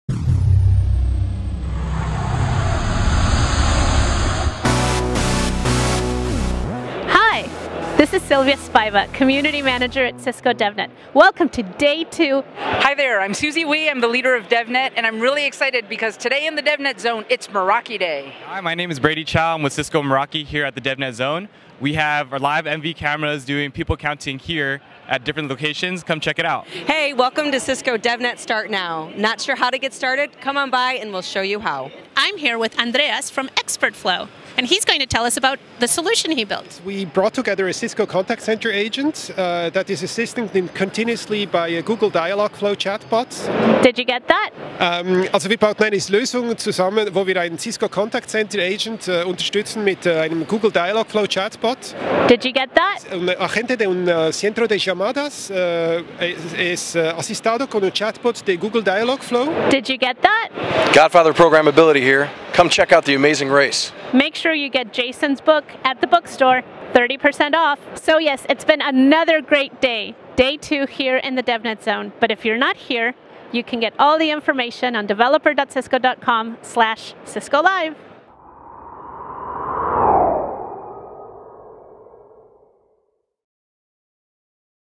Did You Get That ? Cisco Live DevNet Zone Barcelona 2019 - Day 2
‪From a Meraki takeover of the DevNet Zone, to a trip to the Cisco Store, it was a fun day 2 at Cisco Live Barcelona.

‪Have you read Jason Gooley’s Programmability book?

Interview by Silvia Spiva
Video by Chris Oggerino
Magic by Julio Fernandez